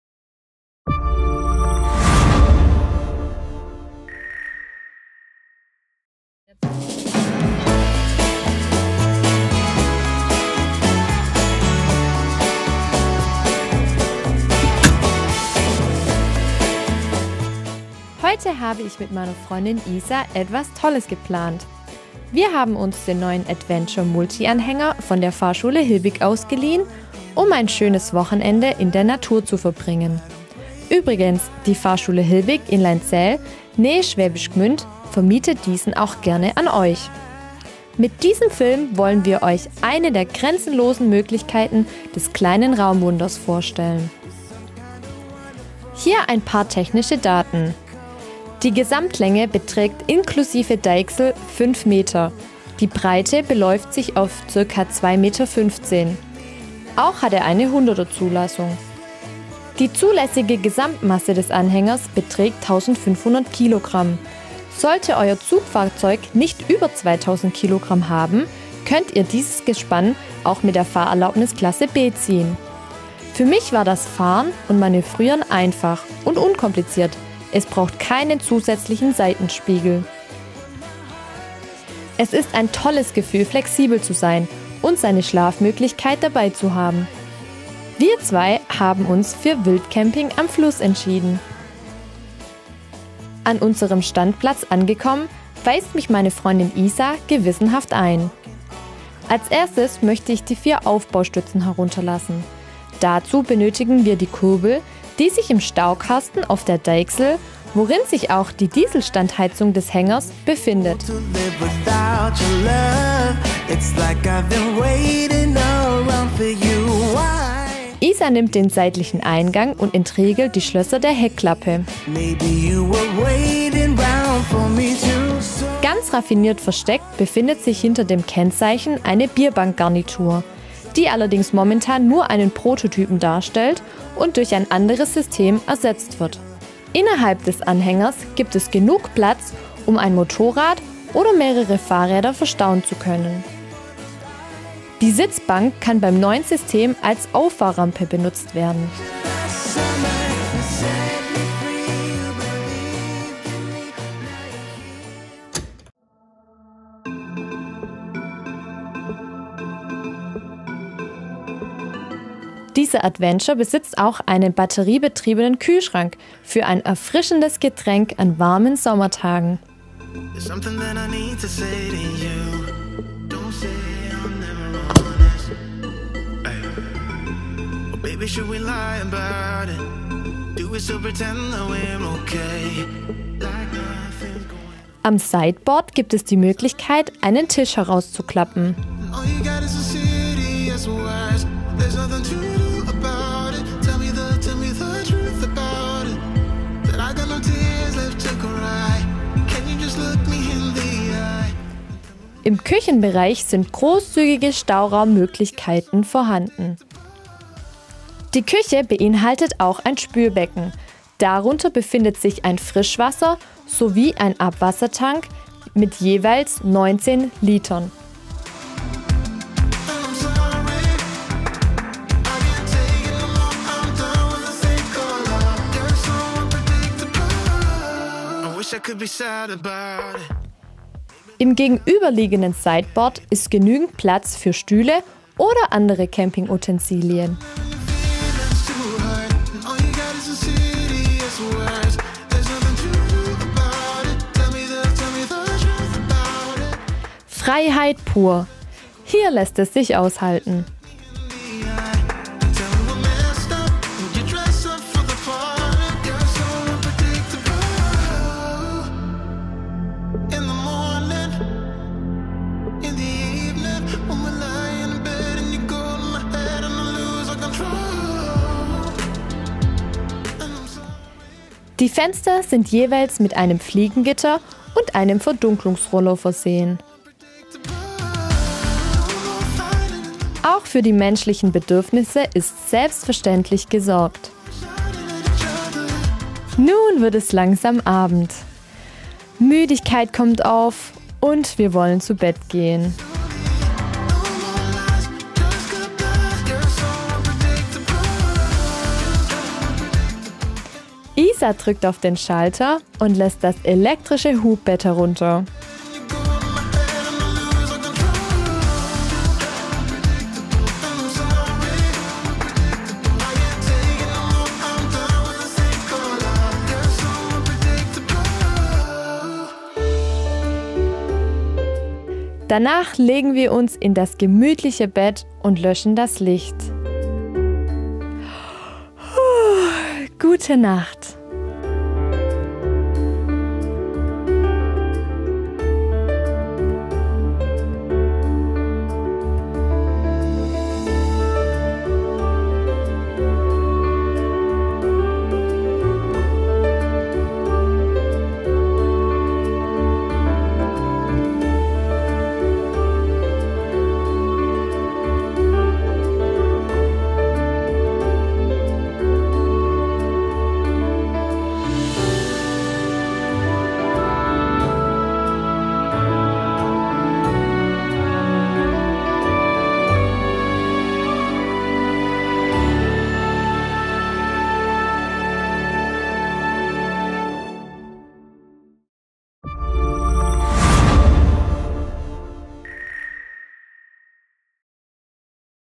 Zu vermieten Wohn- Kofferanhänger ADVENTURE Fahrschule Hilbig thecuber cuber
In diesem Film zeigen uns Nicole und Isa, wie sie ein Wochenende mit dem ADVENTURE erlebt haben. Mit dem Wohnanhänger ADVENTURE können problemlos Motorräder oder auch Fahrräder mitgenommen werden. Ein grosses elektrisches Hubbett wird von der Decke heruntergelassen und gilt als komfortable Schlafstätte. Eine Küche mit viel Stauraum bietet genügend Platz für Ihre Utensilien. Ein Waschbecken und eine Toilette gehören ebenso zu diesem kleinen Raumwunder. Sogar eine Biertisch Garnitur, die auch als Auffahrrampe für die Motor- oder Fahrräder dient ist mit dabei. Lust auf ein Wochenende oder mehr ? Dein Abenteuer kann schon jetzt beginnen. Ein Anruf genügt und Du startest ins Adventurefieber